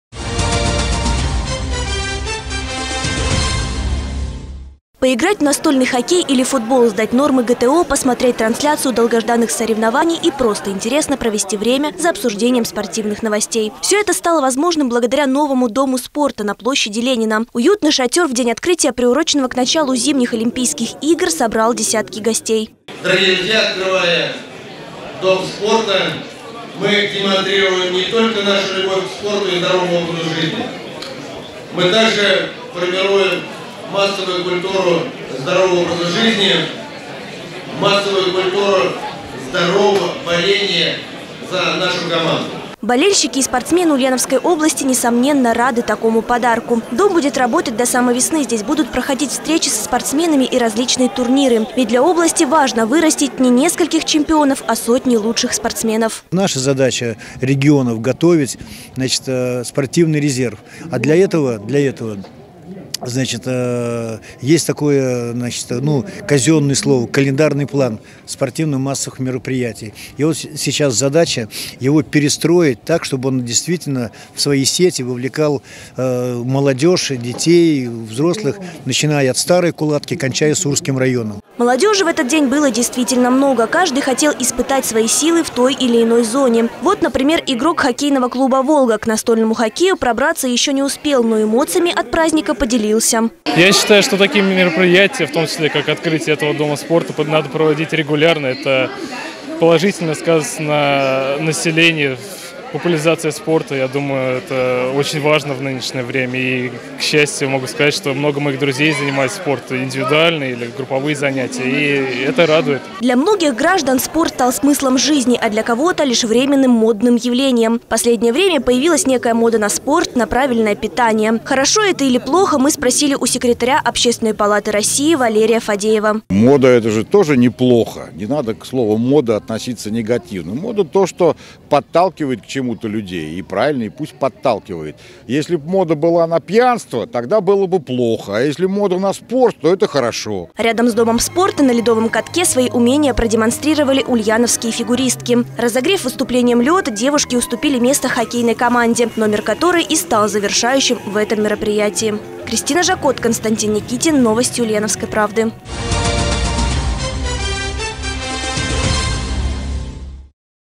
Валерий Фадеев: «Мода на спорт – это здорово!»
Поиграть в настольный хоккей или футбол, сдать нормы ГТО, посмотреть трансляцию соревнований и просто интересно провести время за обсуждением спортивных новостей. Всё это стало возможным благодаря новому Дому спорта на площади Ленина.